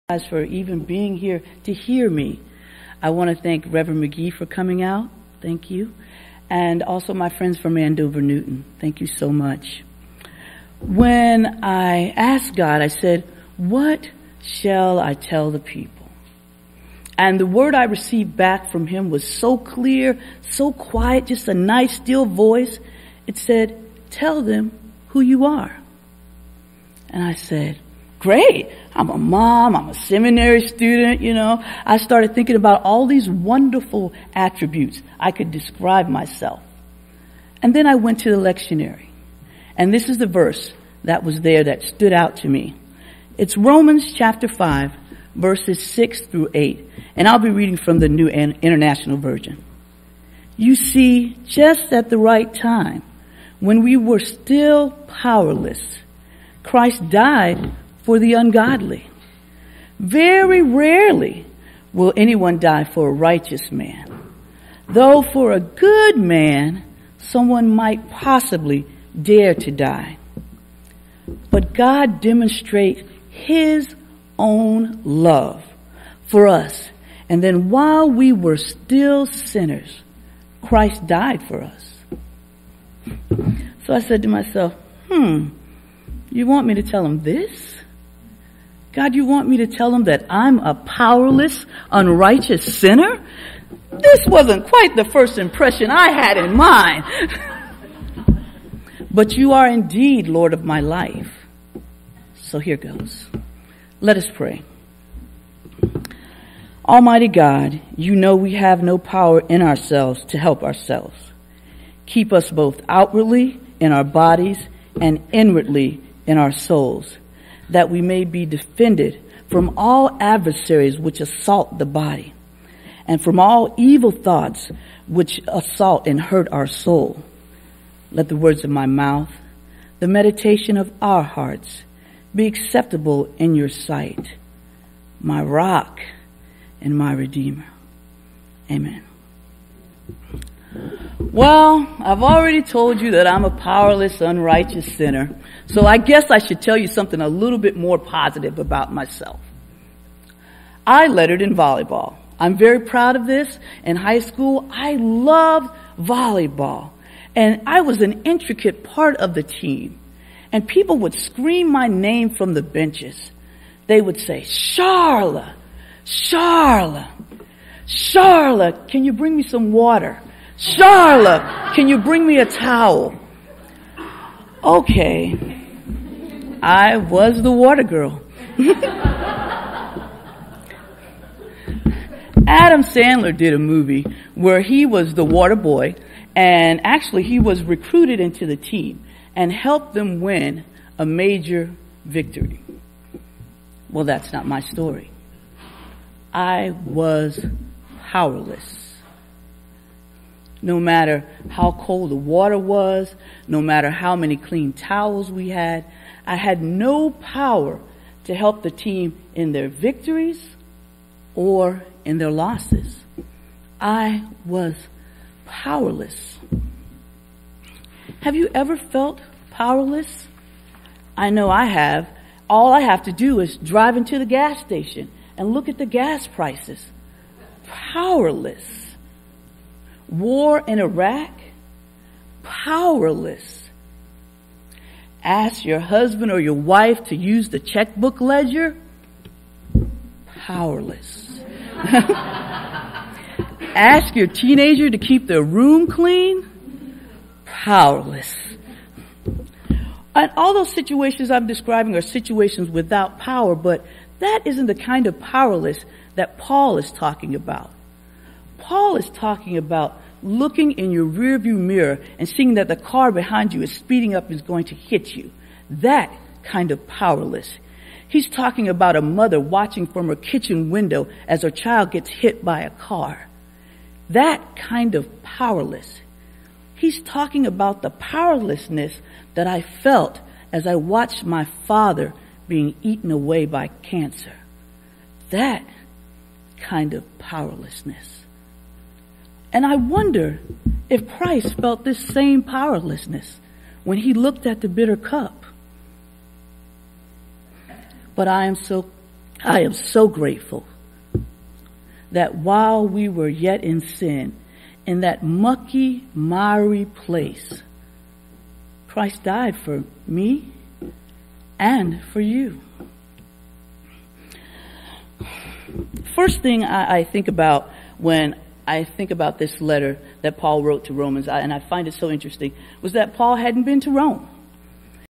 Min. Kouadio
Part one of my first sermon at First Baptist in Needham